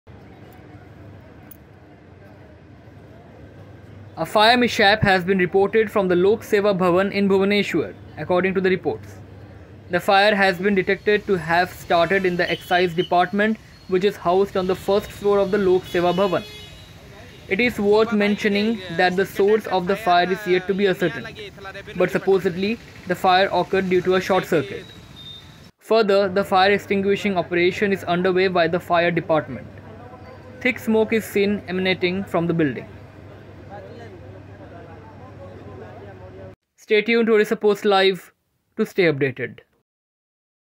Fire mishap at Lokseva Bhavan
A fire mishap has been reported from the Lok Seva Bhawan in Bhubaneswar, according to the reports. The fire has been detected to have started in the Excise Department, which is housed on the first floor of the Lok Seva Bhawan.